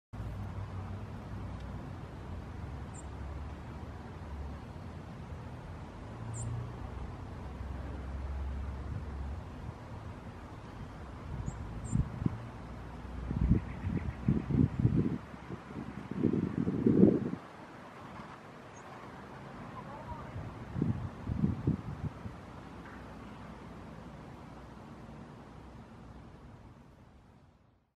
Golden-Crowned Sparrow
A Golden-crowned Sparrow (Zonotrichia atricapilla) in Delta, British Columbia. The black and gold head stripes on the Golden-crowned Sparrow are what primarily determine a bird's status among its wintering flock.